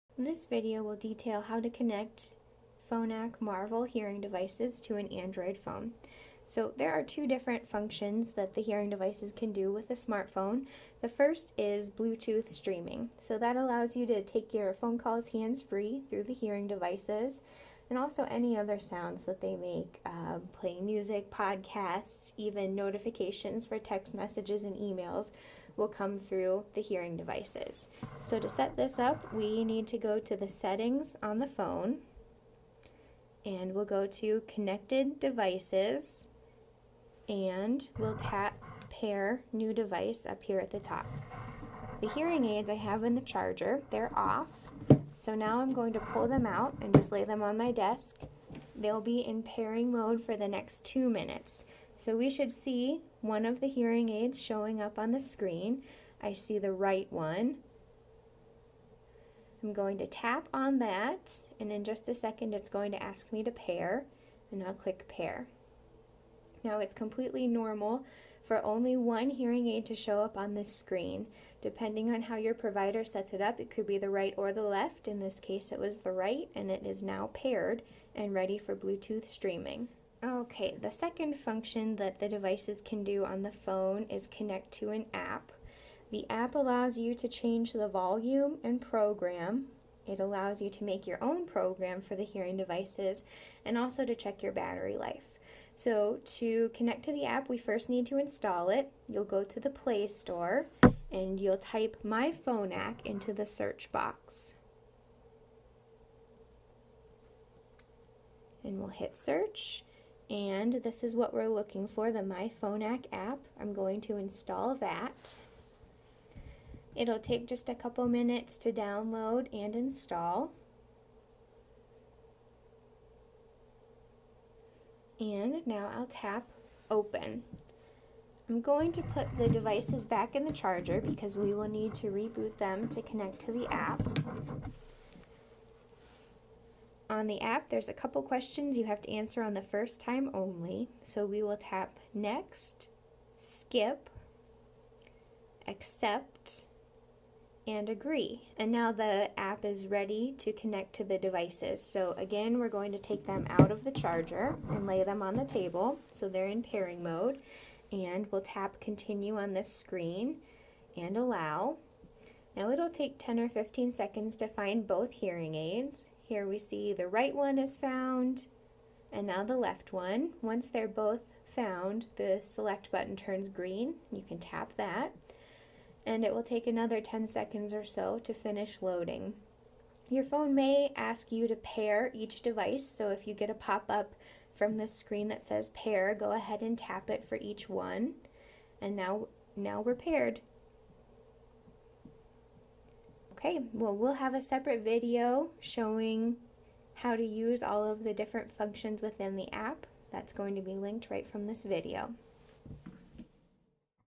Connecting Phonak Marvel hearing aids to an Android phone
Follow along as our audiologist walks you through the process of connecting an Android phone to Phonak Marvel hearing aids.